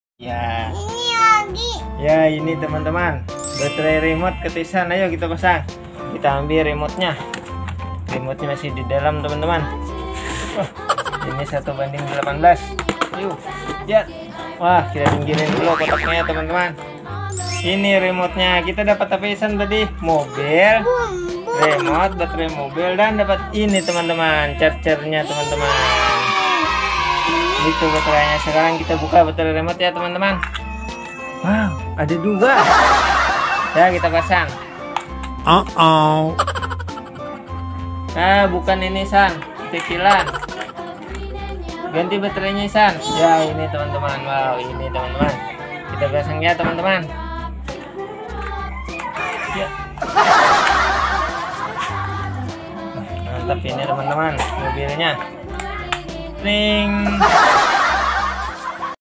Unboxing mobil remot offroad pick up part3
dalam video Unboxing mobil remot offroad pick up part3 bercerita tentang M. ihsan kamil dan Rizka aulia lagi unboxing mobil remot offroad pick up murah berkualitas dengan harga cuma 130 ribuan dalam bentuk video shorts.

untuk rc pick up yang proponya kalian bisa lihat video unboxing rc pick up full propo murah berkualitas di

semoga video Unboxing mobil remot offroad pick up part3 sangat menghibur kalian.

jangan lupa tonton juga ya video